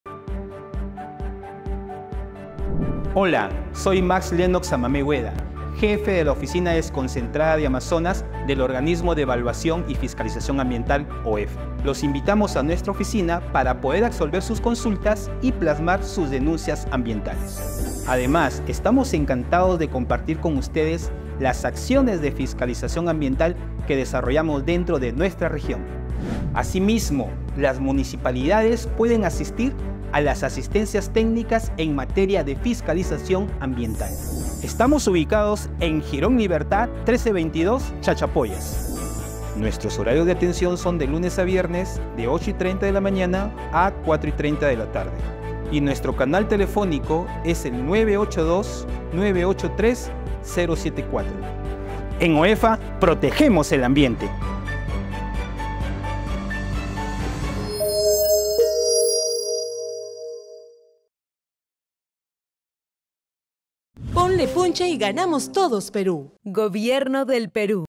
Conoce +: Amazonas
Conoce + sobre la función de las oficinas desconcentradas del OEFA 🤓
✅ Max Samamé, jefe de la Oficina Desconcentrada de Amazonas, nos explica un poco más sobre el tema.